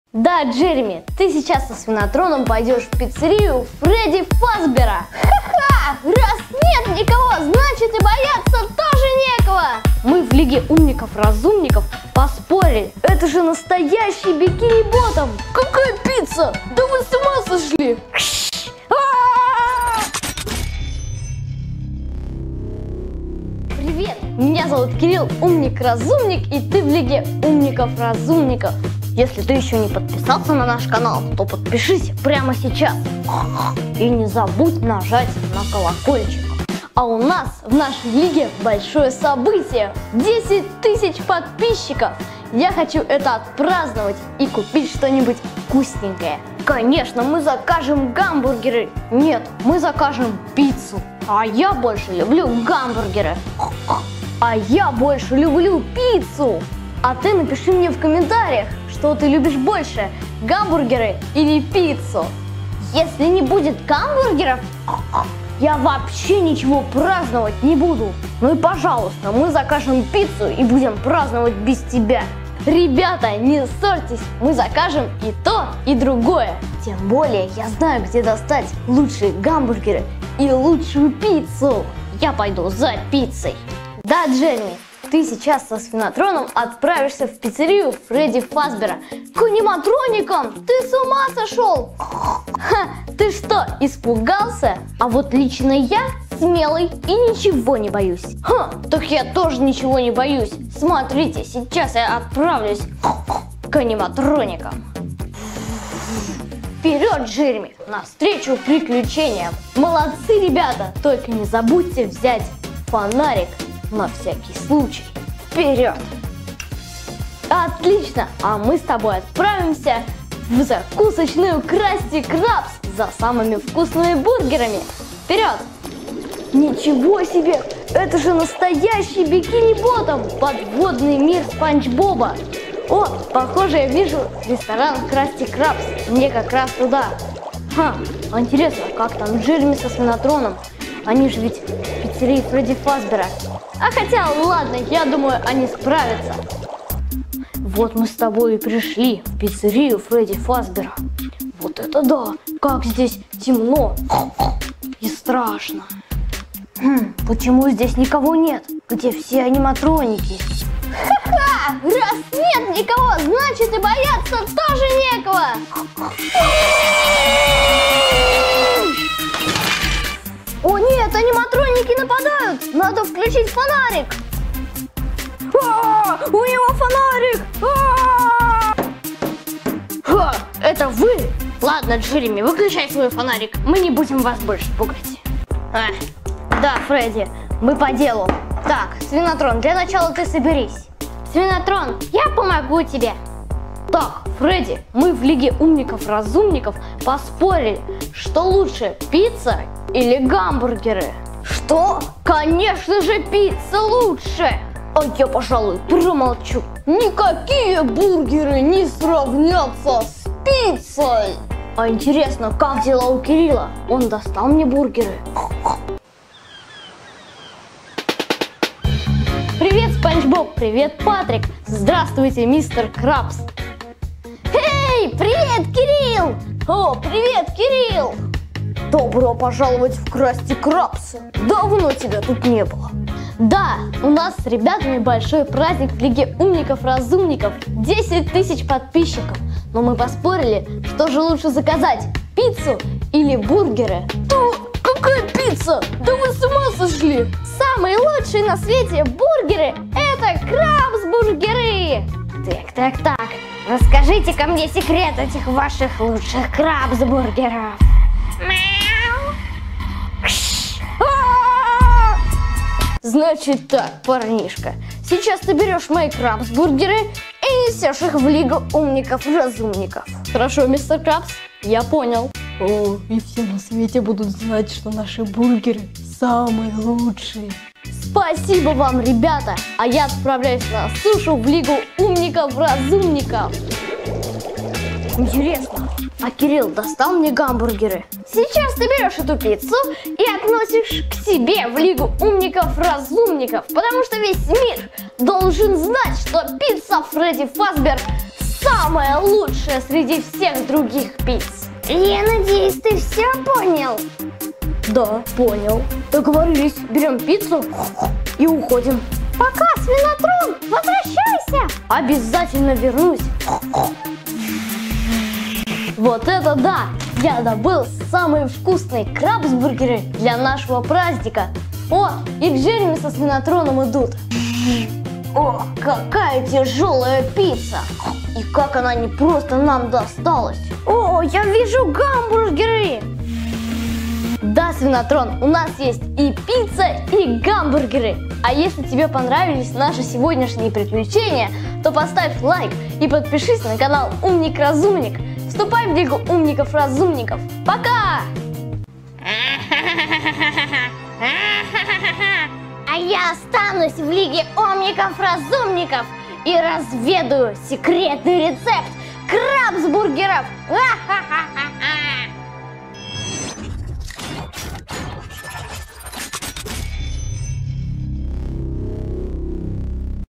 FREDDY FAZBEAR'S PIZZA против КРАСТИ КРАБС отмечаем 10000 подписчиков! Кирилл Джереми и Аниматроники
Я очень рад, что мы вместе! И с каждым днём нас становится всё больше! Наша Лига Умников-Разумников растёт - нас уже больше десяти тысяч!
Сегодня мы станем свидетелями спора между Джереми и Свинотроном - что же лучше? - гамбургеры или пицца?! В этом нам помогут разобраться Фредди Фазбер из Пиццерии FREDDY FAZBEAR'S PIZZA и Мистер Крабс - владелец Закусочной КРАСТИ КРАБС и начальник Спанч Боба - тот самый Губка Боб Квадратные Штаны из Бикини Боттом! Давай, посмотрим, чем закончится этот спор!

И, конечно, жми "колокольчик", чтобы первым узнавать о новых видео на канале Умник-Разумник!